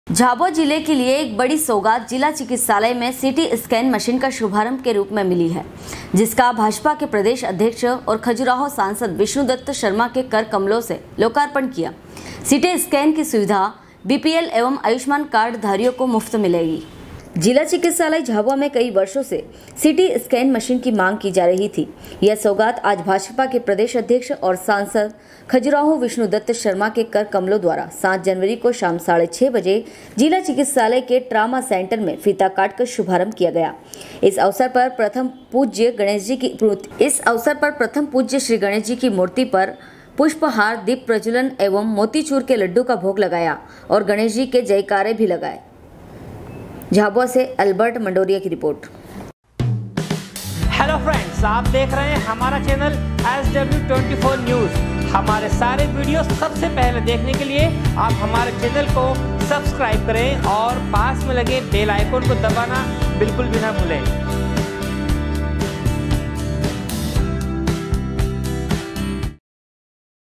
SW 24 News Madhyapradesh
झाबुआ जिले के लिए एक बड़ी सौगात जिला चिकित्सालय में सीटी स्कैन मशीन का शुभारंभ के रूप् में मीली है, जिसका भाजपा के प्रदेश अध्यक्ष और खजुराहो सांसद,विष्णु दत्त शर्मा के कर कमलों से लोकापर्ण किया, सीटी स्कैन की सुविधा बीपीएल एवं आयुष्मान कार्ड धारियों को मुफ्त रहेगी। झाबुआ से अल्बर्ट मंडोरिया की रिपोर्ट।